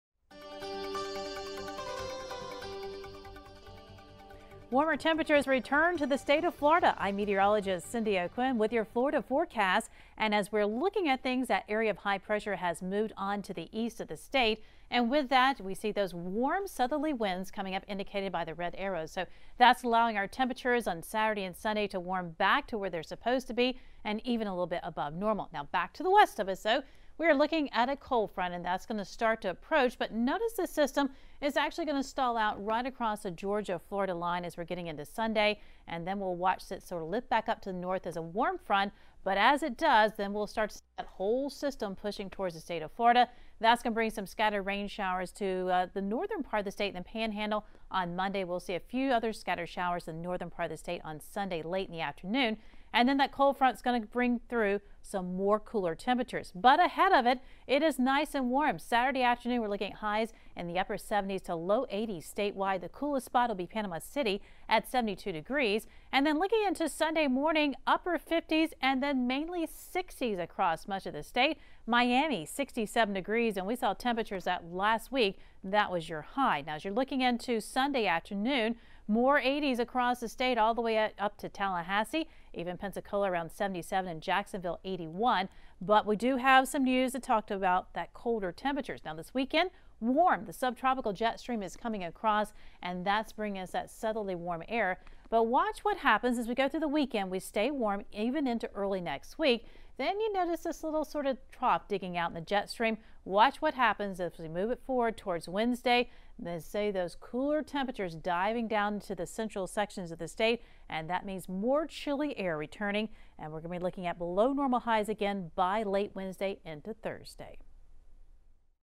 12 PM Mar 16, 2018: "Weekend Warm-up" by Meteorologist Cyndee O'Quinn
Local forecast as seen on WRUF-TV (Cox 1006 or 10.1) and published Friday Mar 16, 2018 at 12:31:10 PM.